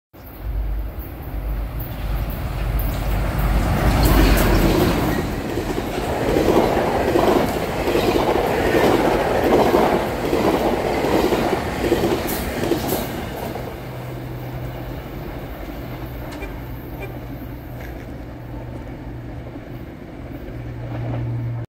(Equipment Move) (10 Cars) (Mid Old Cab Car 231) GO E-1325 - 642 With 372 At Mimico
(Service Operation)

Terminated: Oakville GO Station

(Equipment Move)

Originated: Oakville GO Station